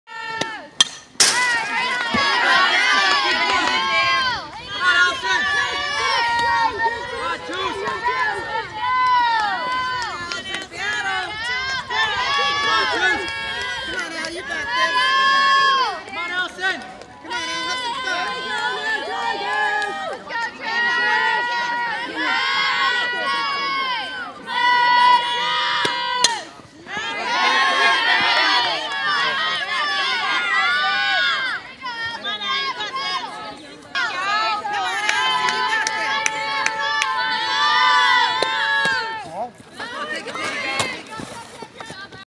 Softball
Taunton High takes on Dighton-Rehoboth